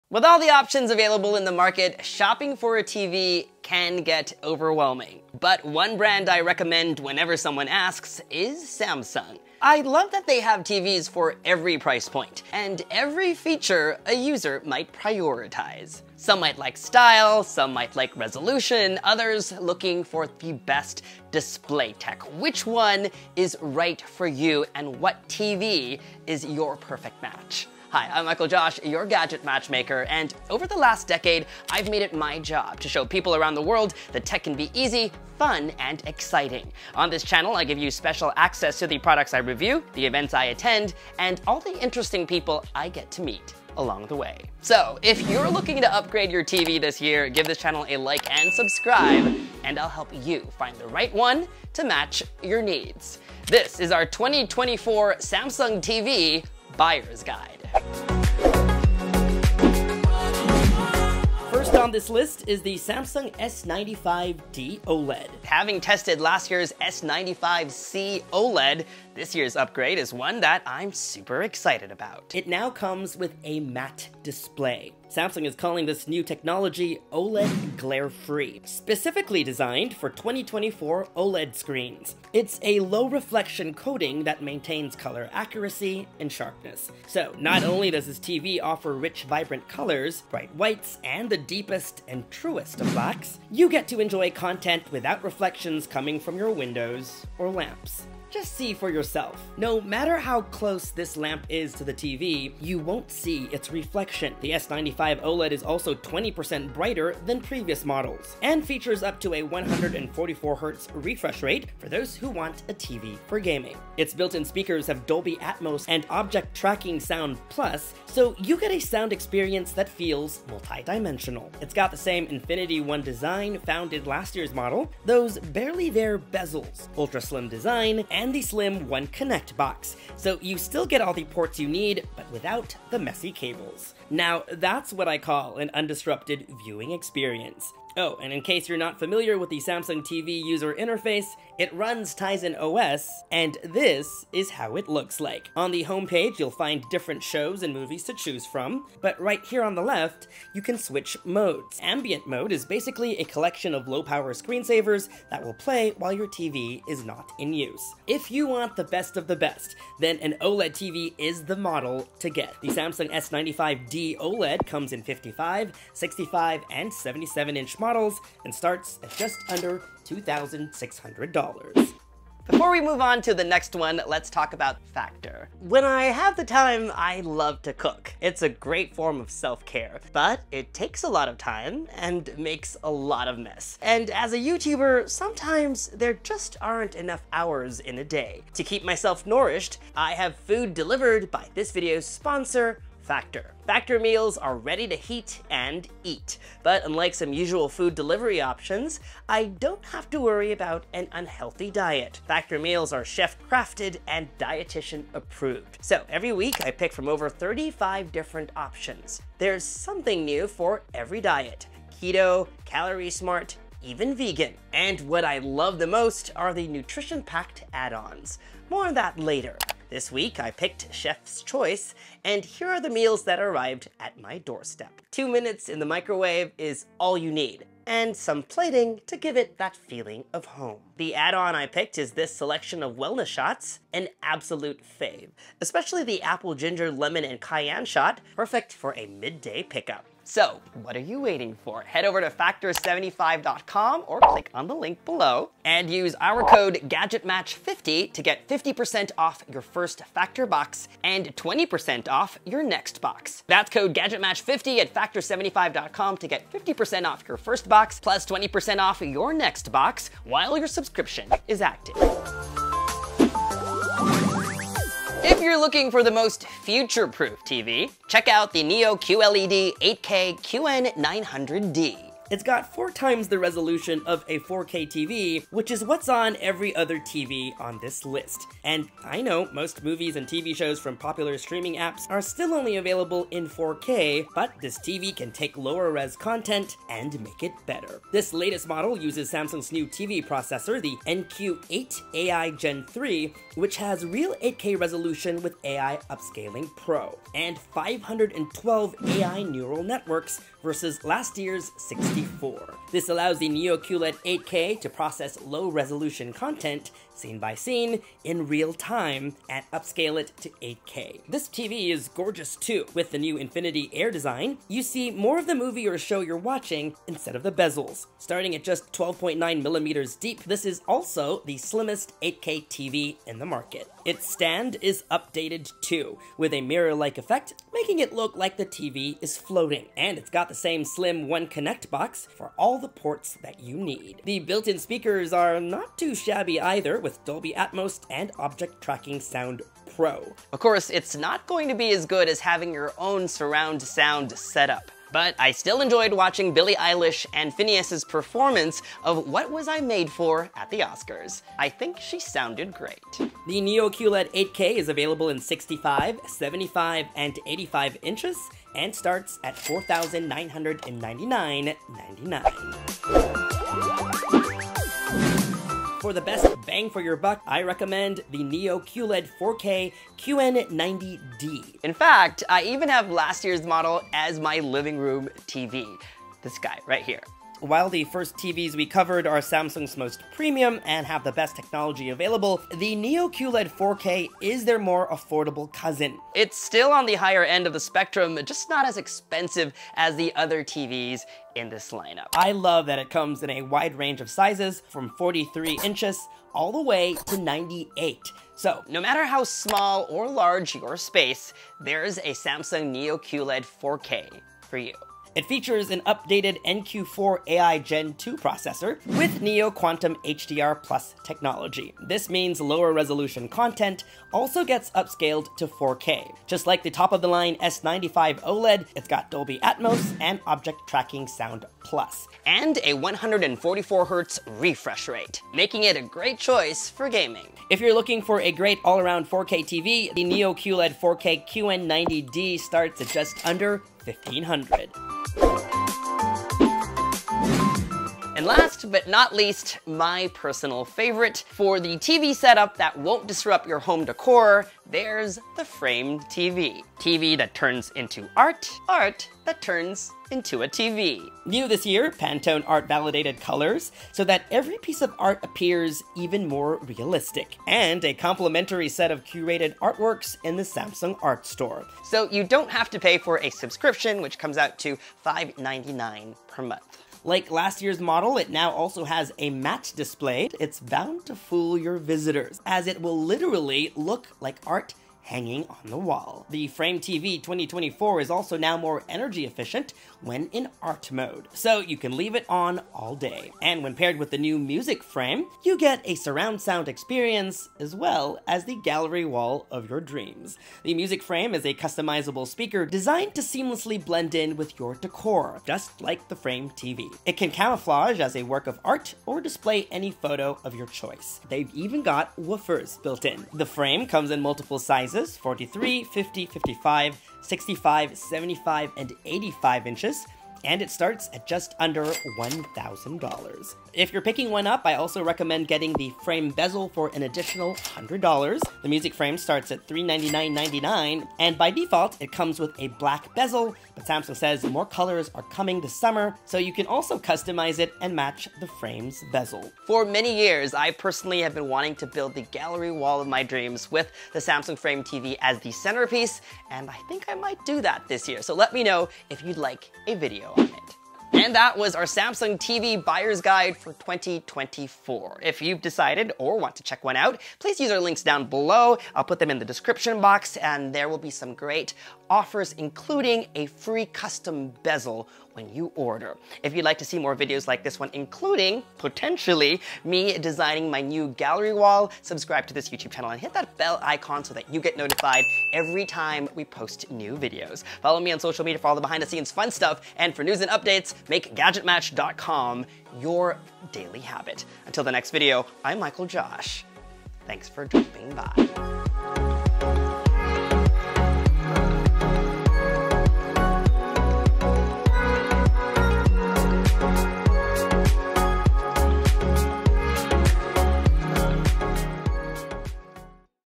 Samsung TV Buyer's Guide 2024: Which One Should You Get?
With all the options available in the market, shopping for TVs can get overwhelming. One brand I recommend whenever someone asks is Samsung. I love that they have TVs for every price point and every feature a user might prioritize. Which one is right for you? Keep watching our 2024 Buyer's Guide to find out.

CHAPTERS:
00:00 Intro
00:57 S95 OLED
02:54 Factor
04:21 Neo QLED 8K QN900D
06:02 Neo QLED 4K QN90D
07:14 2024 The Frame
09:00 Thanks For Dropping By!

My Camera (body)
My Everyday Lens
My Favorite Lens
My Tripod
My Video Head
My Travel Video Head